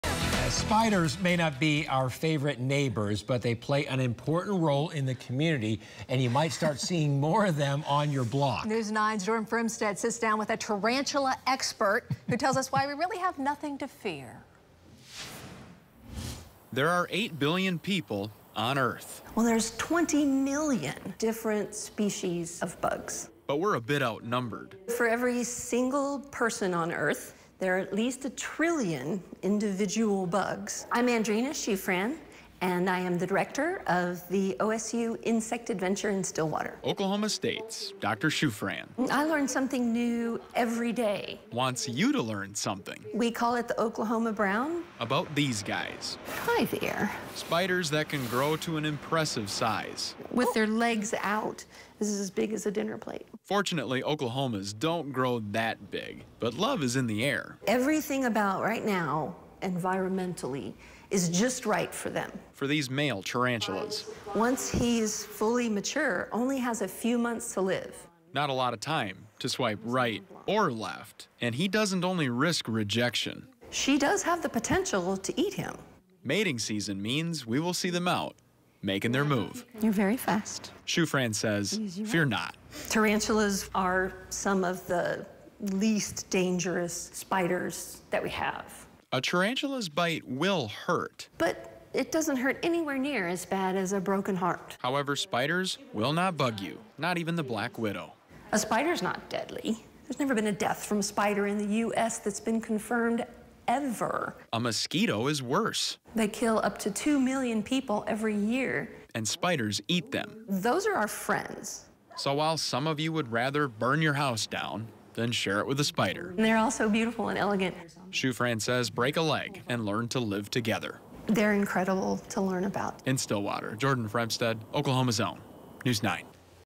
Expert Dispels Myths About Tarantulas Amid Mating Season
A spider-expert shares why these crawly creatures actually do more harm than good, despite their bad rap.